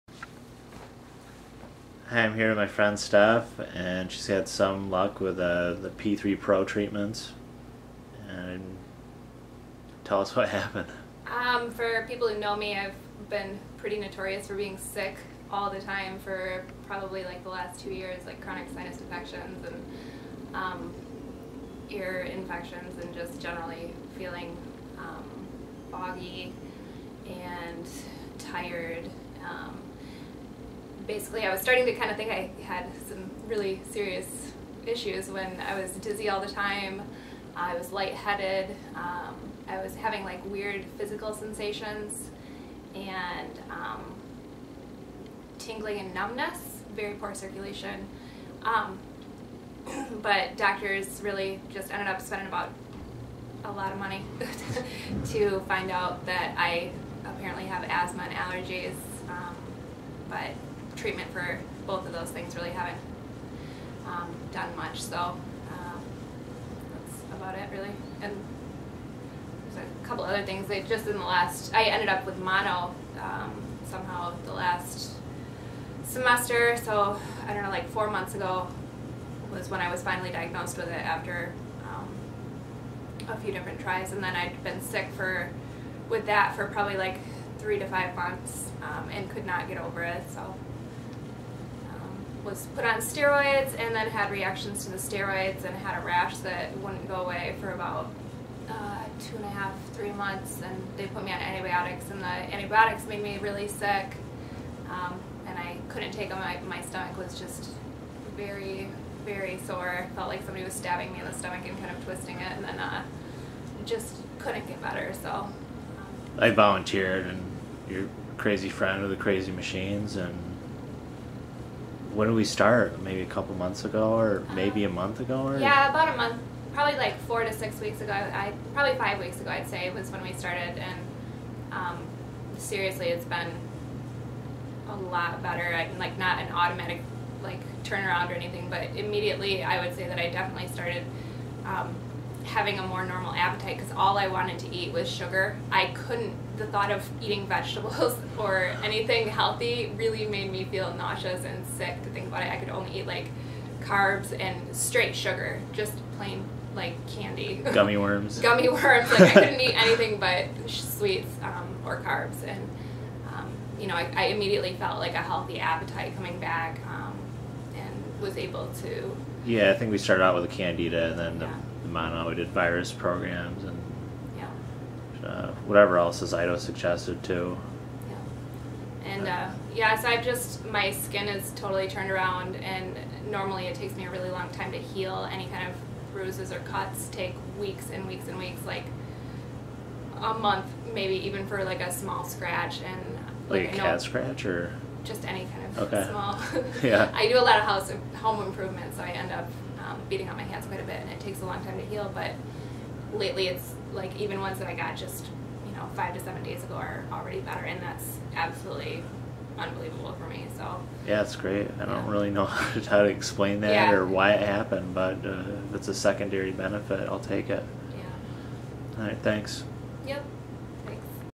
Accelerated wound healing, Mononucleosis crushing, Candida remediation
p3pro treatment of candida, mono and accelerated wound healing